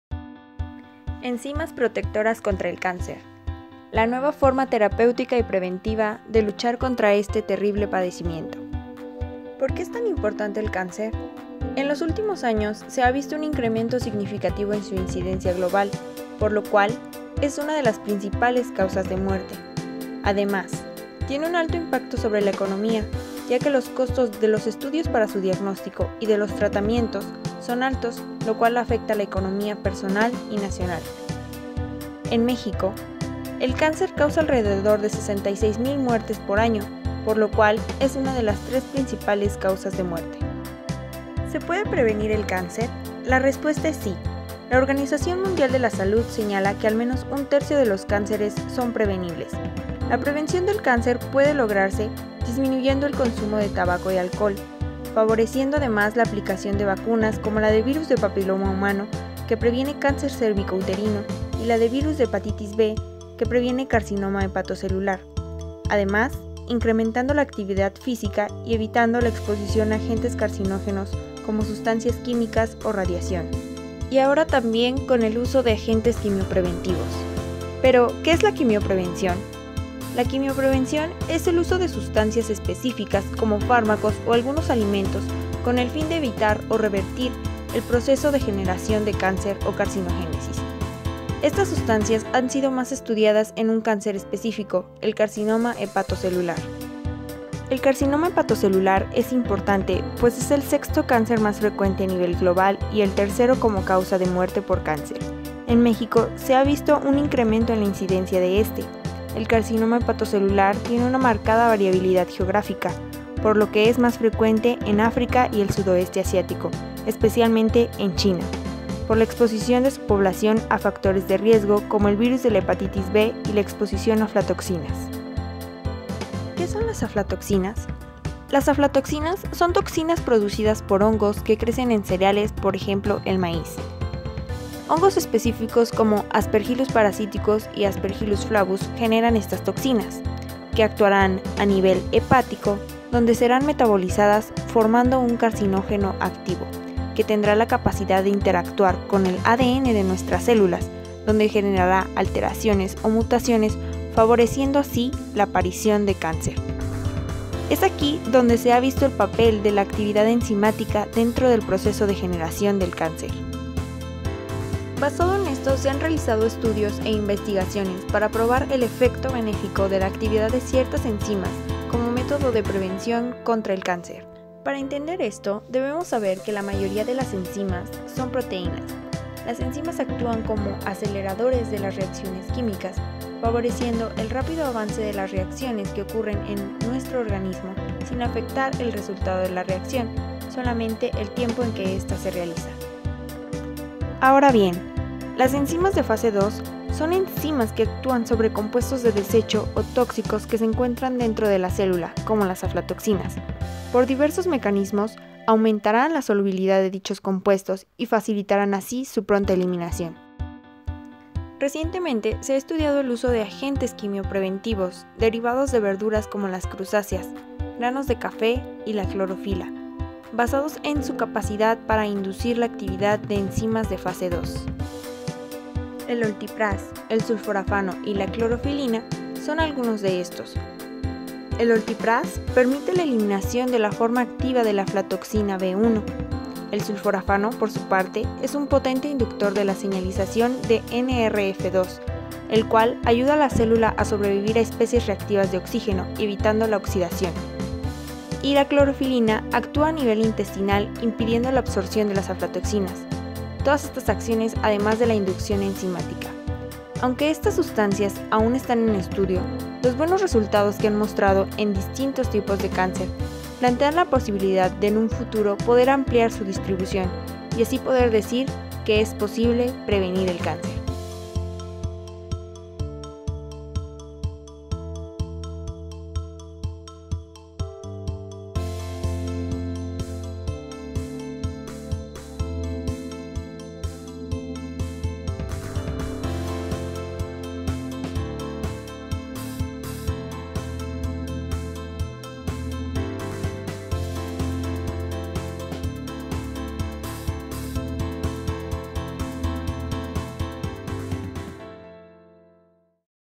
ENZIMAS, EL FUTURO CONTRA EL CÁNCER
In recent decades, various investigations have focused on discovering a way to prevent cancer. These investigations have studied the substances that act by inducing certain enzymes in our body, which favor the elimination of carcinogens. These have been studied more thoroughly in hepatocellular carcinoma, in populations that are constantly exposed to substances called aflatoxins, found in certain foods, which have a direct effect on the appearance of cancer.